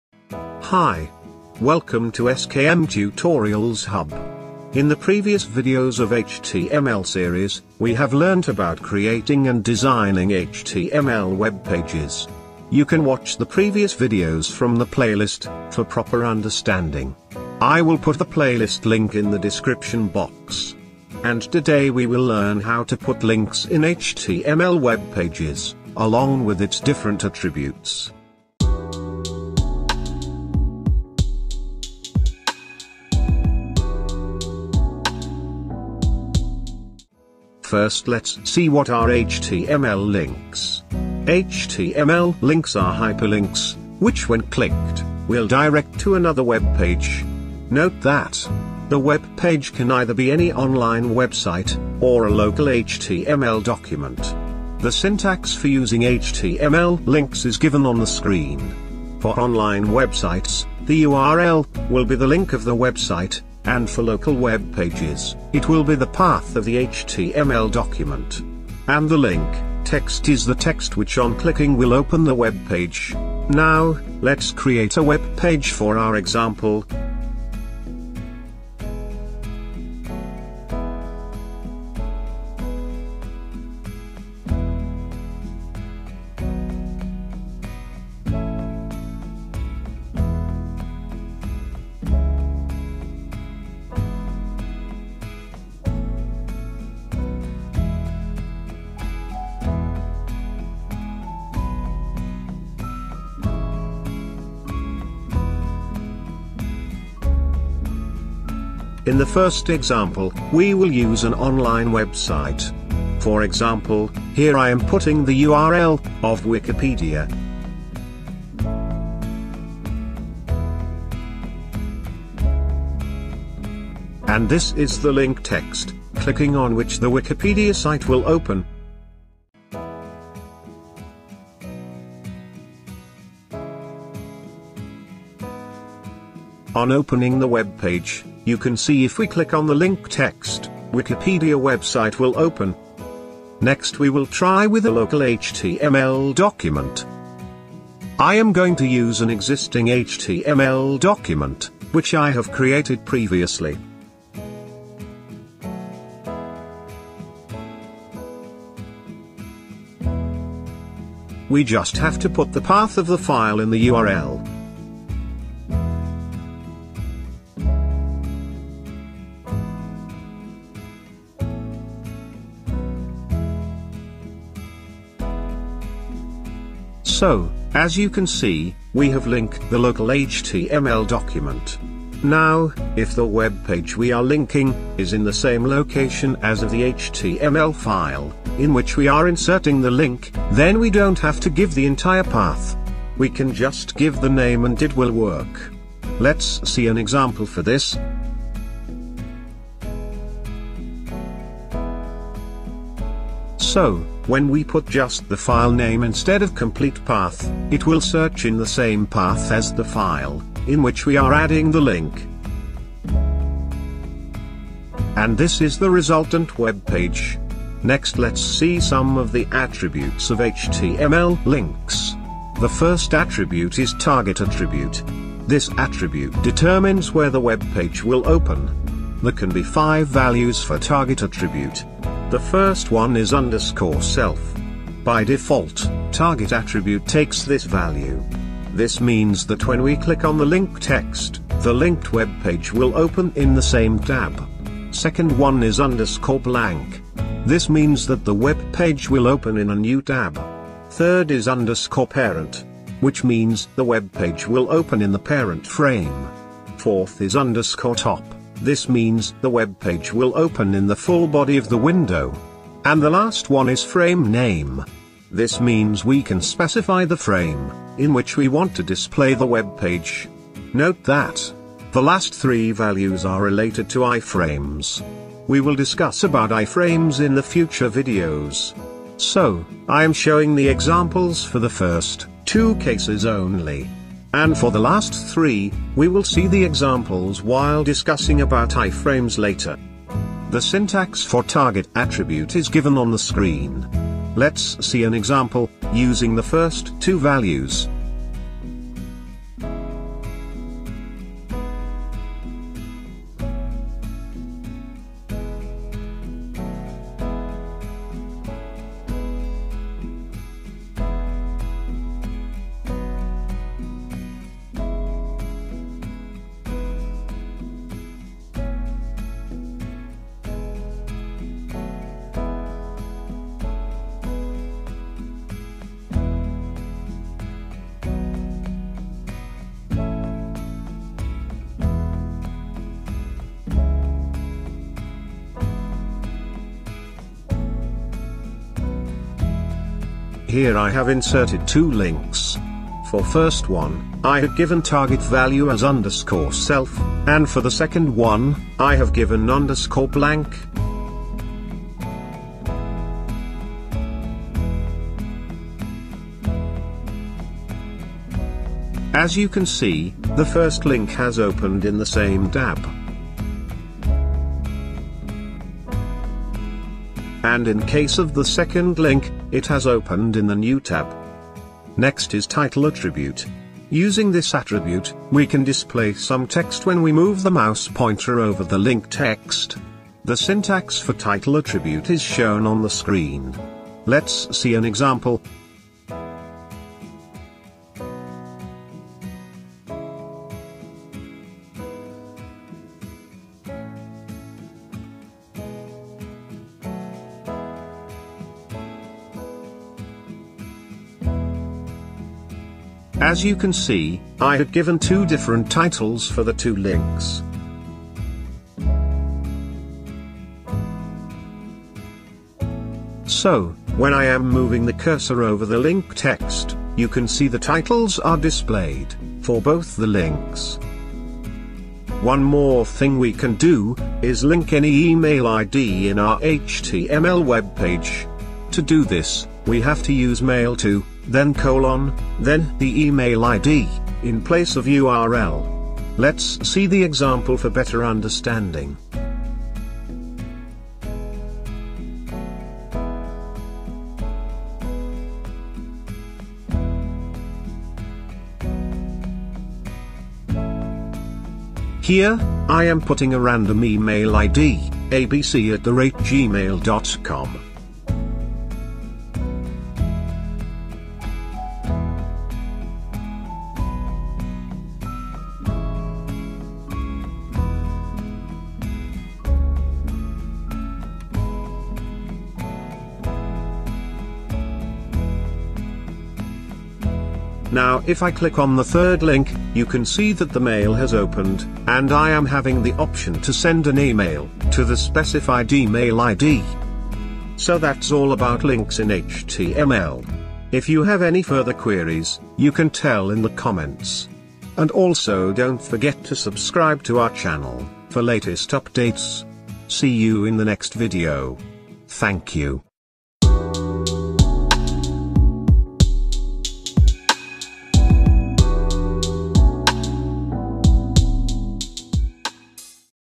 How to insert links in HTML webpage | HTML tutorials for beginners
Learn how to insert links in your HTML webpage, what are its different attributes and how to use them using examples.

Topics discussed in this video :

What are html links
How to insert links in html
Target attribute in HTML links
Title attribute in HTML links